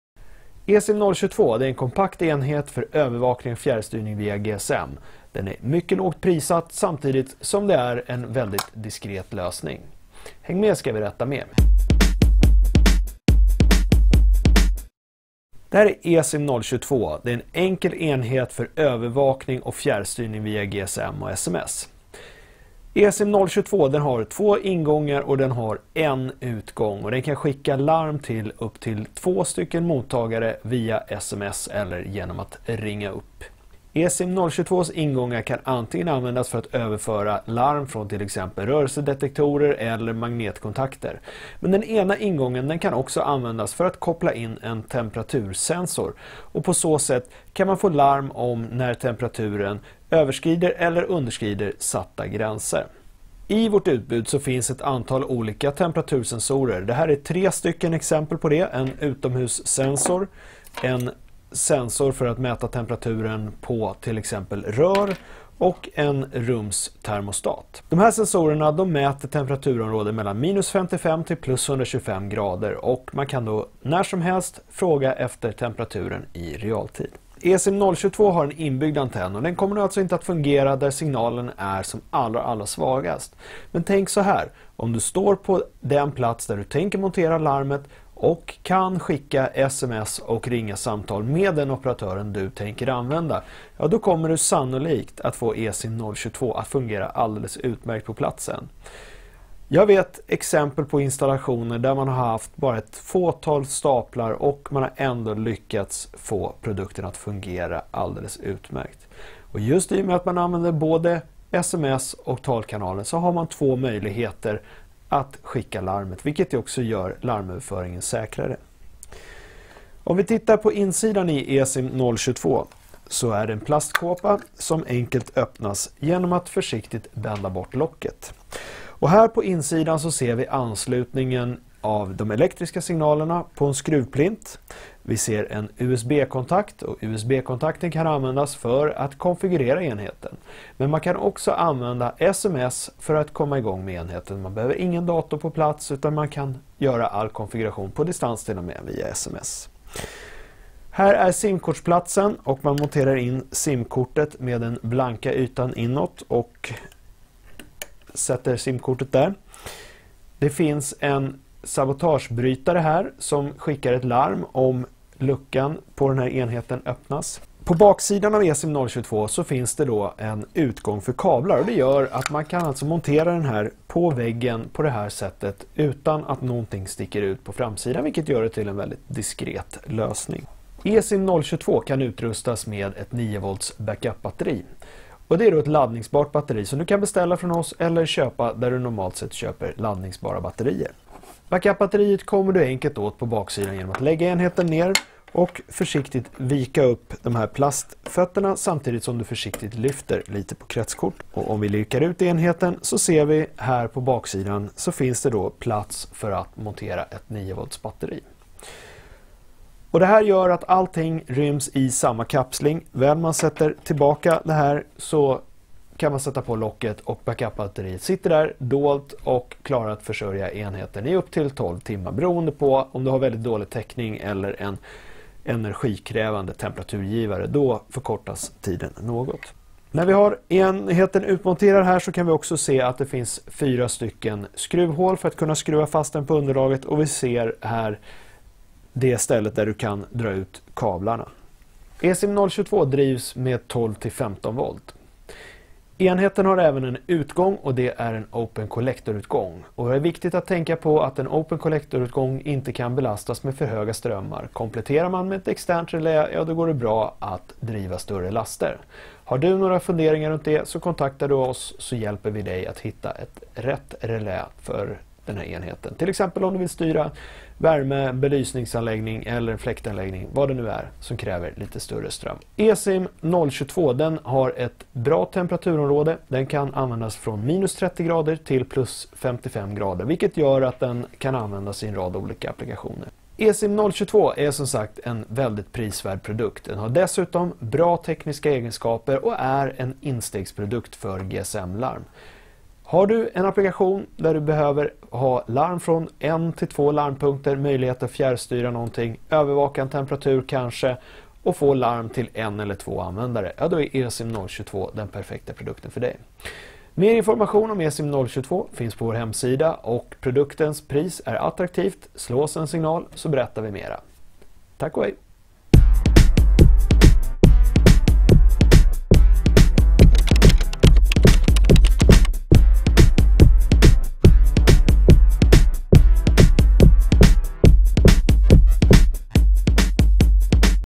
ESIM022 kostnadsefffektiv GSM-larmlösning
GSM larm med möjlighet att övervaka temperatur eller styra en utgång. Inbyggd batteribackup och smidig idriftsättning. En diskret GSM-larmslösning som sätter en ny ribba för GSM-larm, både avseende funktion och pris. Läs mer på